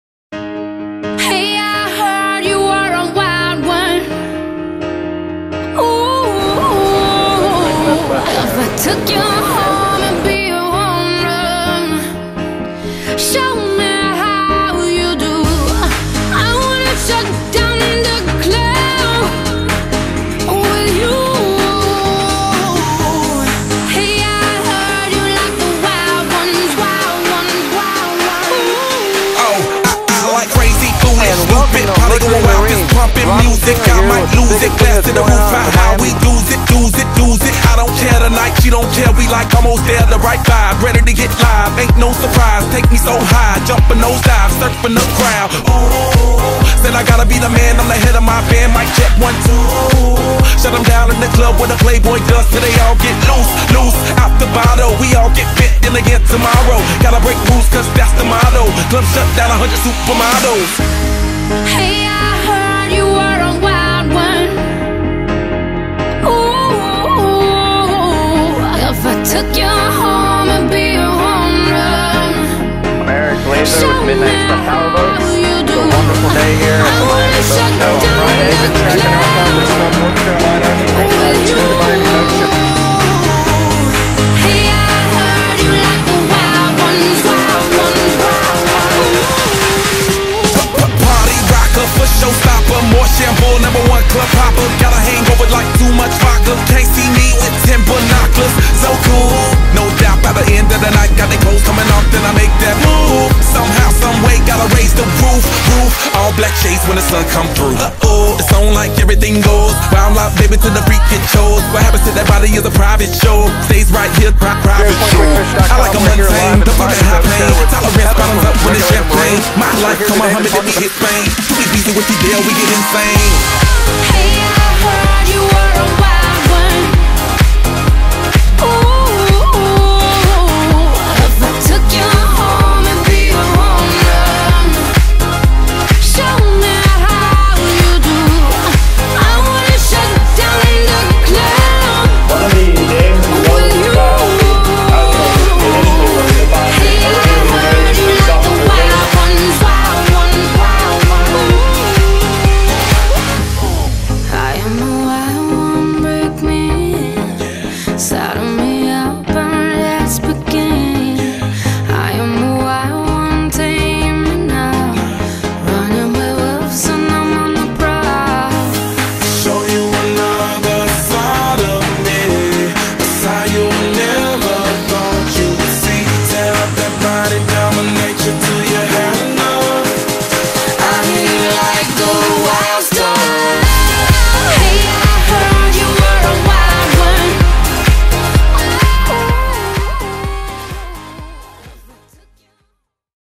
PointClickFish.com 2013 Miami Boat Show Coverage Highlight Video
The PointClickFish.com Team covered the 2013 Miami Boat show and posted images, BLOGS, videos, and LIVE streaming content from the show and marina.